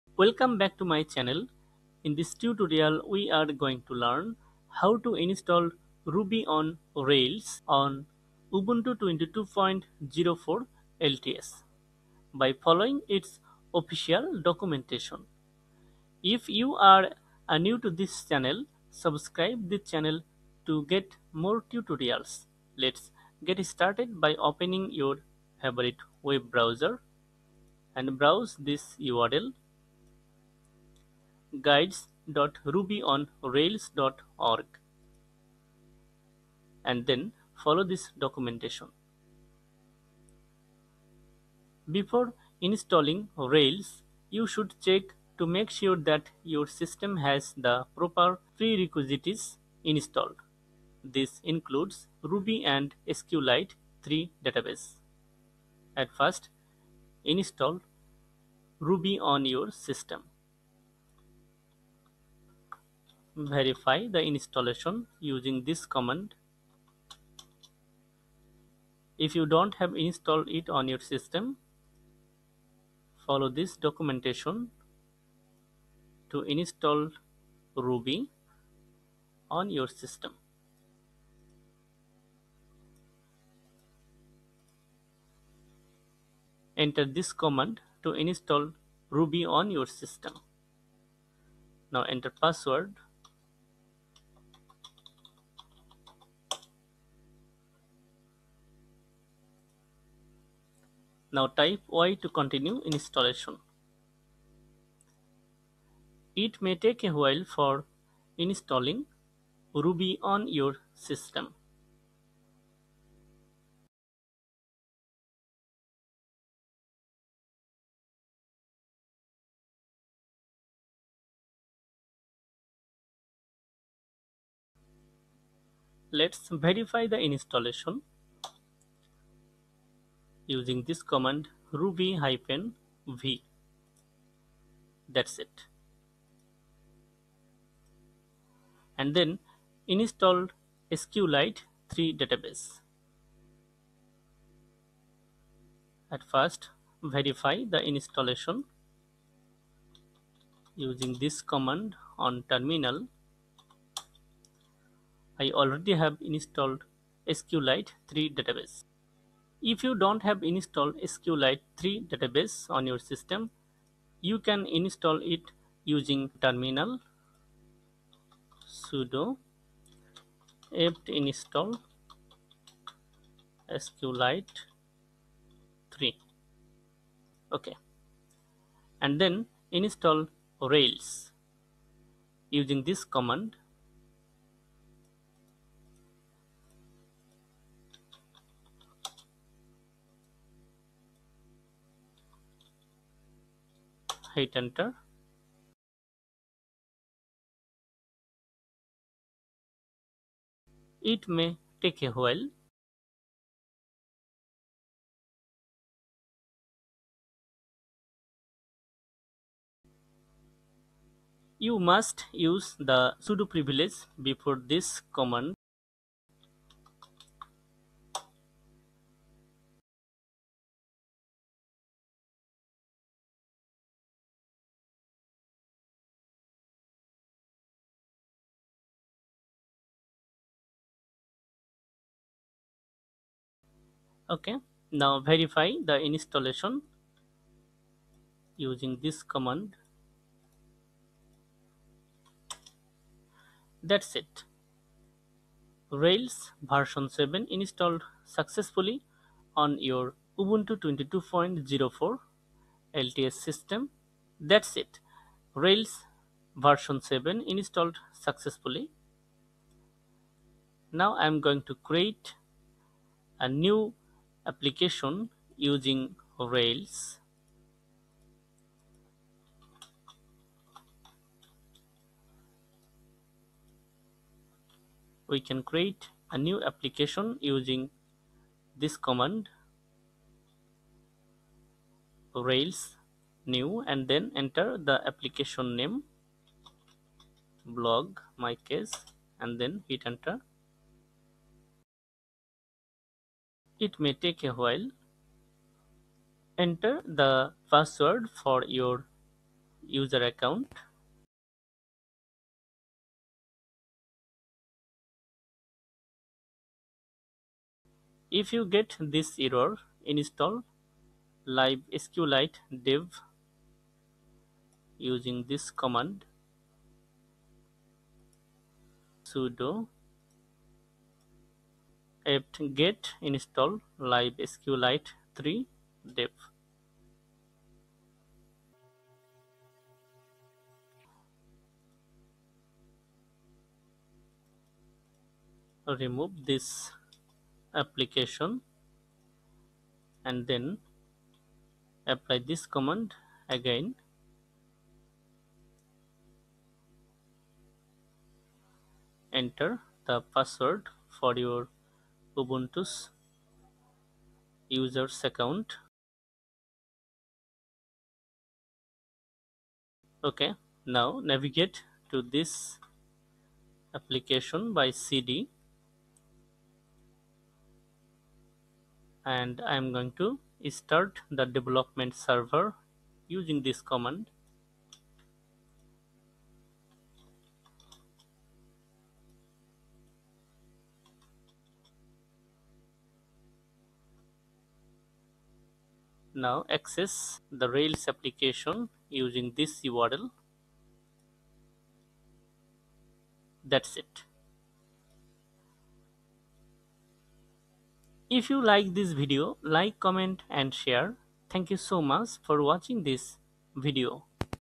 How to install Ruby on Rails 7 in Ubuntu 22.04 LTS (Linux)
This is a complete tutorial, here, You will get learn How to install  Ruby on Rails 7 in Ubuntu 22.04 LTS and How to create a Rails Application and How to install Ruby in Ubuntu 22.04 LTS and How to install Ruby on Rails 7 in Linux and How to install SQLite3 in Ubuntu 22.04 LTS.